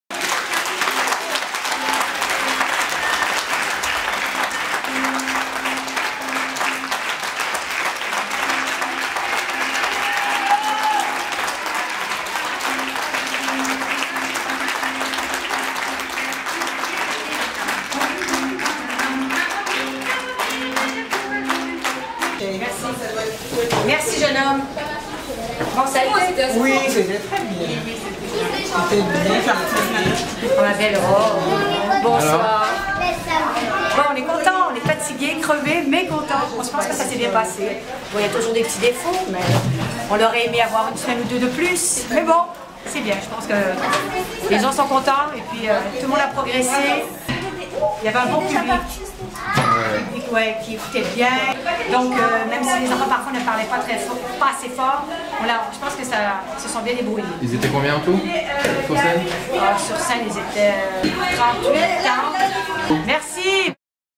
FRIDOLINADES / KORZEMO / vendredi 15 janvier 2016
Vendredi 15 janvier 2016, les différents groupes de théâtre de l'association Korzémo de Ducos se sont produits sur la scène du centre culturel du bourg du Lamentin.
Les Fridolinades, c'est la vie des Québécoises et Québécois durant la période de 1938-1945: la seconde guerre mondiale, le travail des femmes dans les usines, les jeunes conscrits, l'omniprésence de la religion dans le quotidien, la vie des ouvriers de la ville de Montréal...